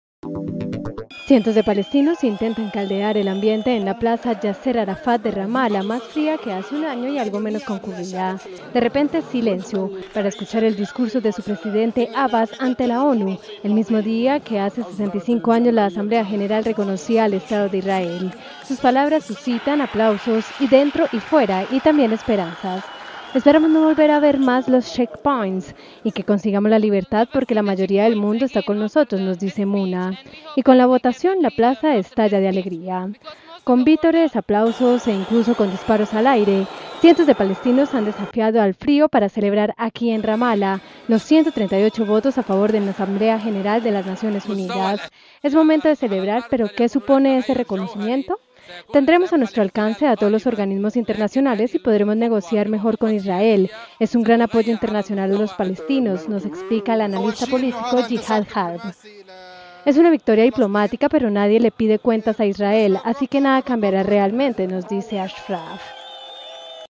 Palestinos celebran el reconocimiento de la ONU
Los líderes libaneses calificaron hoy de "victoria y avance histórico" el reconocimiento de Palestina en la ONU como estado observador no miembro.